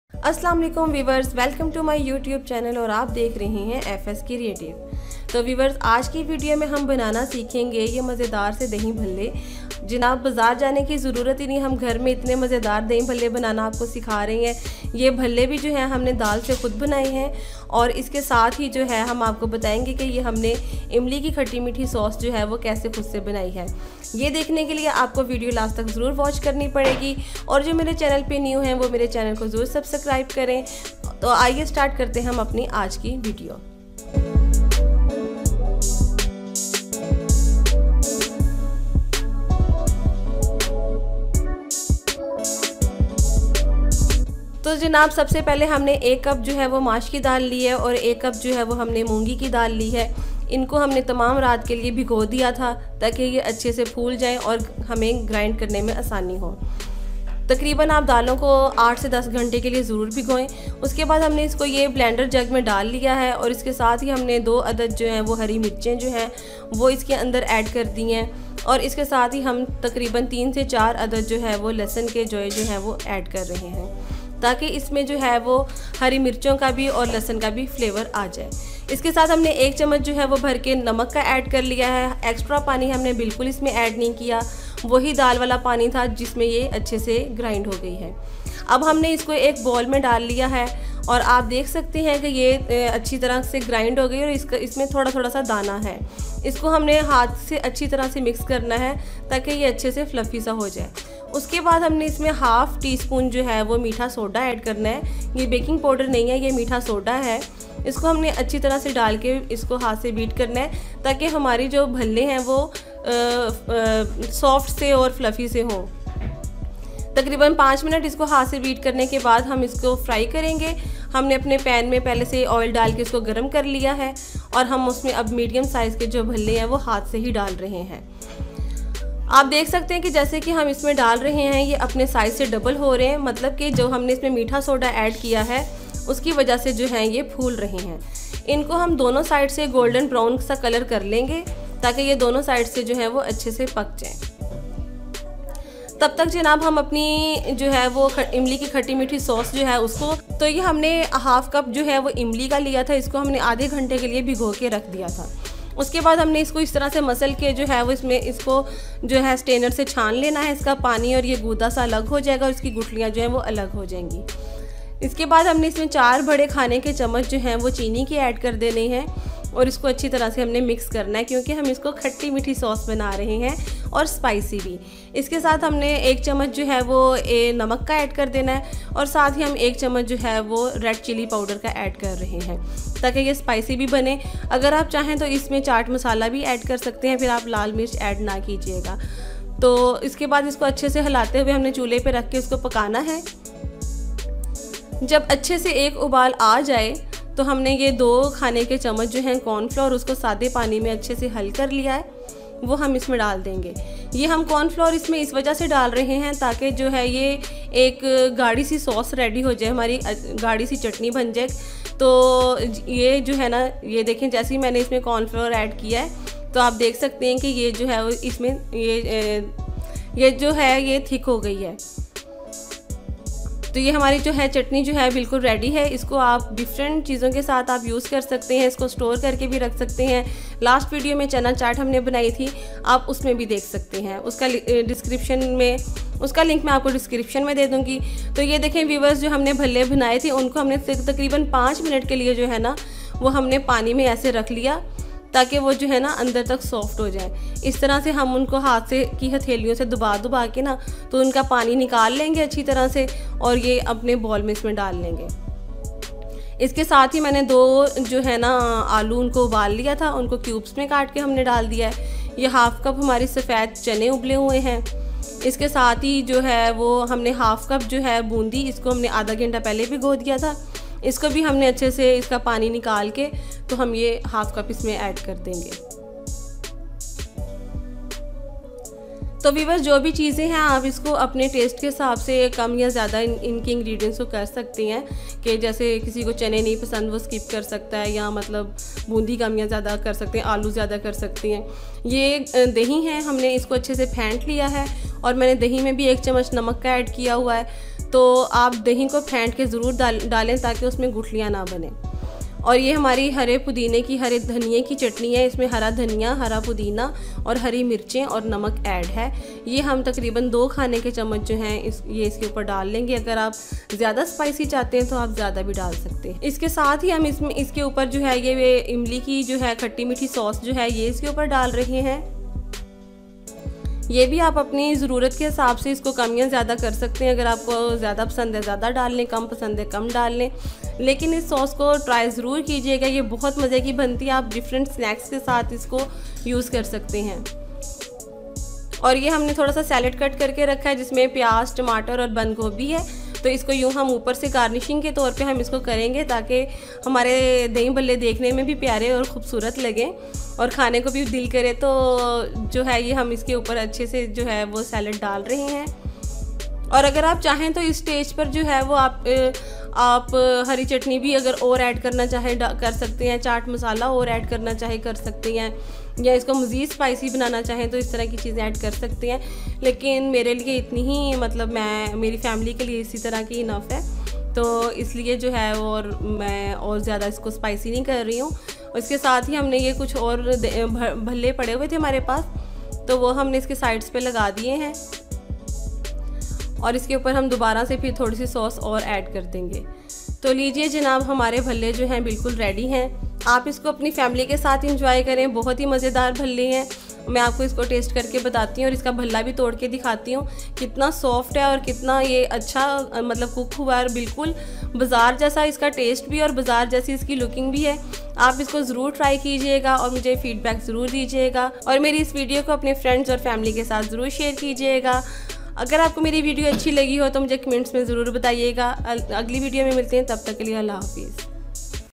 Dahi Bhallaon waly Dal ke Bhally | Dal k Bary Bazar Jise Soft Banain | FS Creative
Watch Video Dahi Bhallaon waly Dal ke Bhally, Dal k Bary Bazar Jise Soft Banain Or Maza Maza Sa Kahiyn.